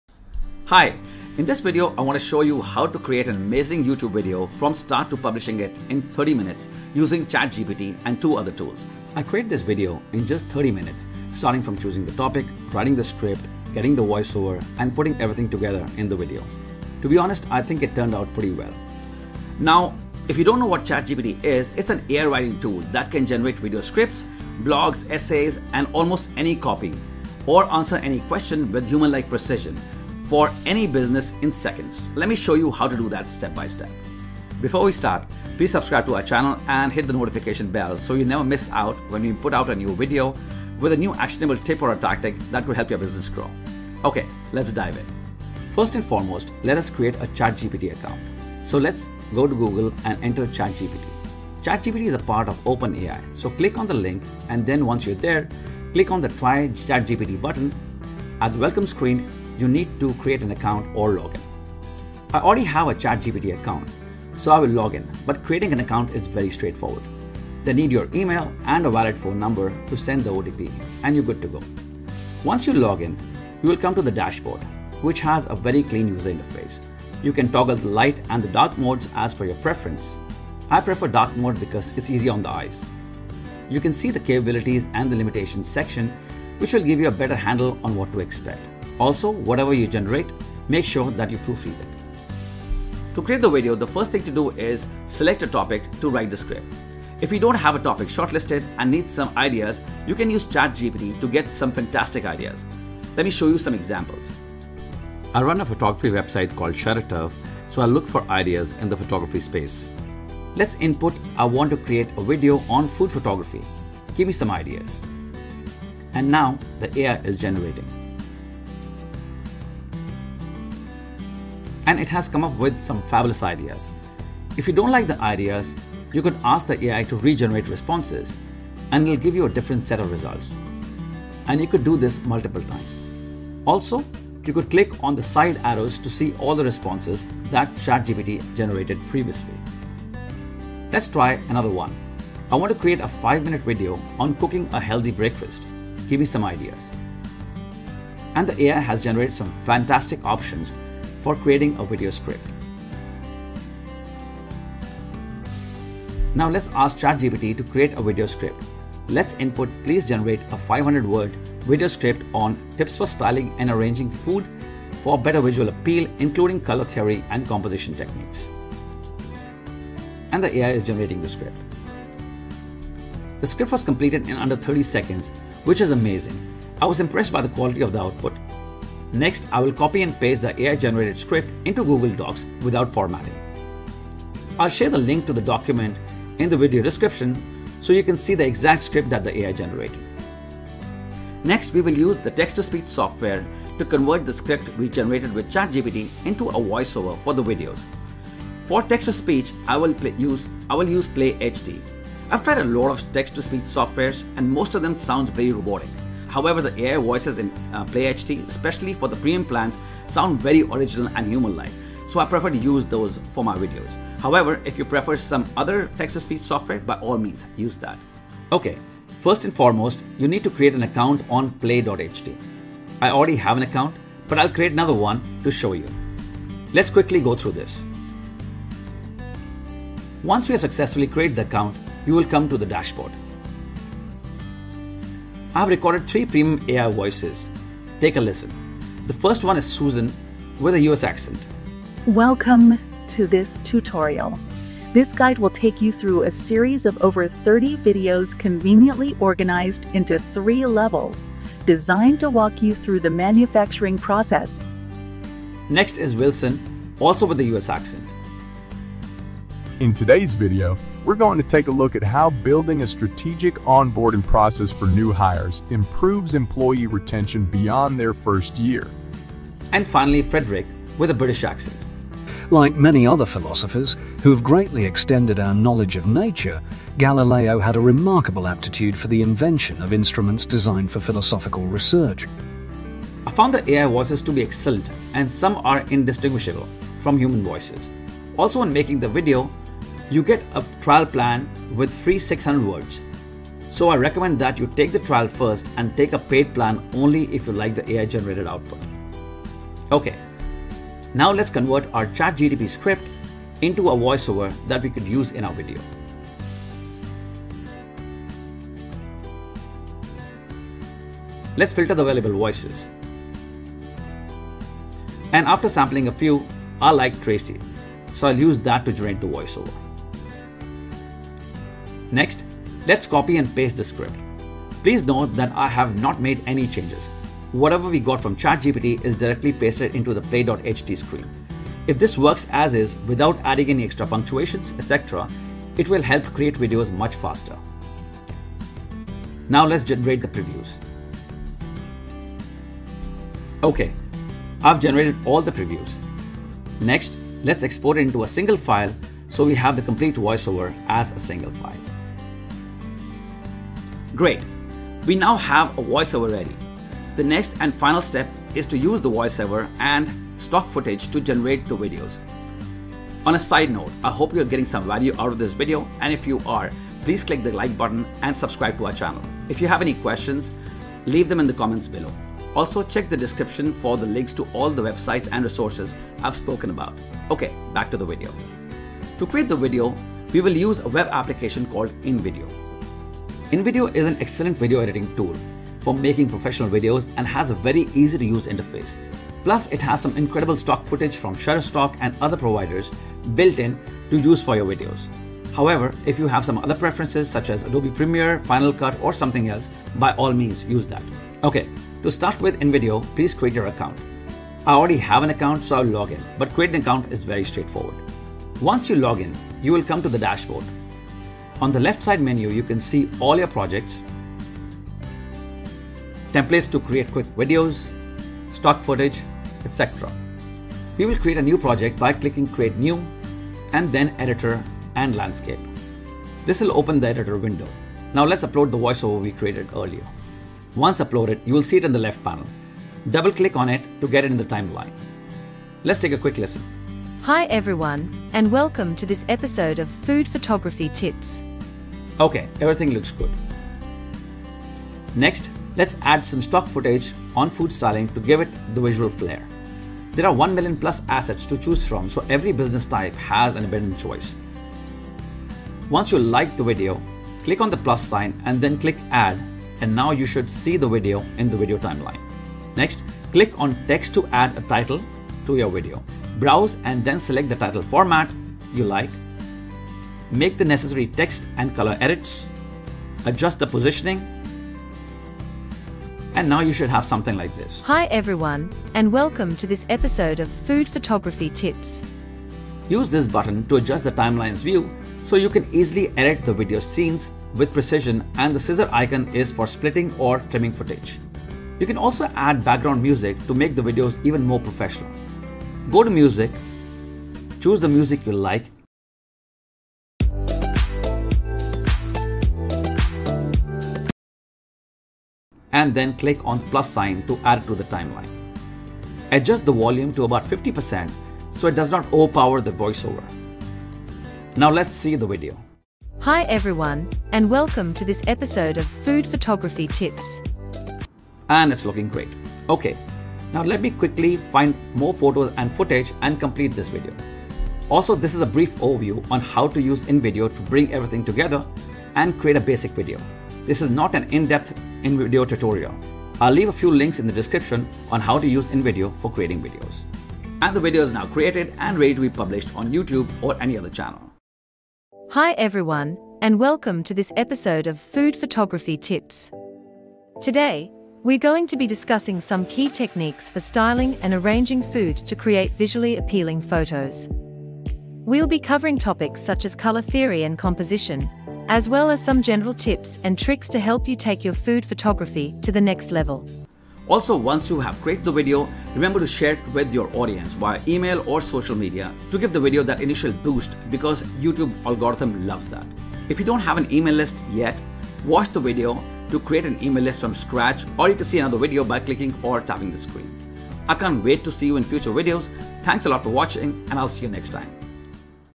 How to use ChatGPT to Create High-Quality AI Videos
Are you looking for a fast and efficient way to create high-quality youtube videos? In this tutorial, we show you how we used ChatGPT, an AI writing tool, to generate ideas and a script for our video.

In addition to ChatGPT, we also used Play.ht, a text-to-speech tool AI, to turn our script into a professional-sounding voiceover. And with InVideo, a video editing tool, we added stock footage and polished the final product. Together, these AI tools made it easy for us to create a high-quality video from start to finish in just 30 minutes.

We hope this tutorial inspires you to try using AI tools like ChatGPT, Play.ht, and InVideo to streamline your own video production process.